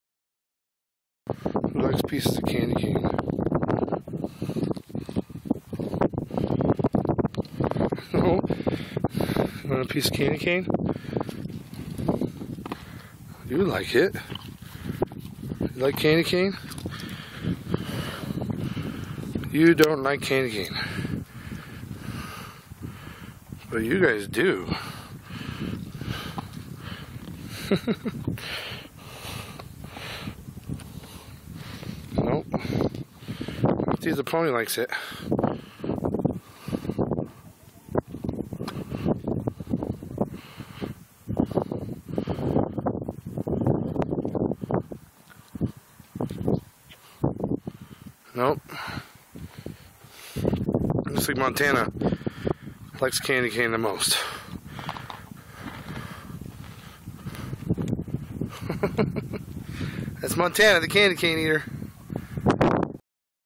Montana likes candy cane
She was the only horse that liked the candy canes.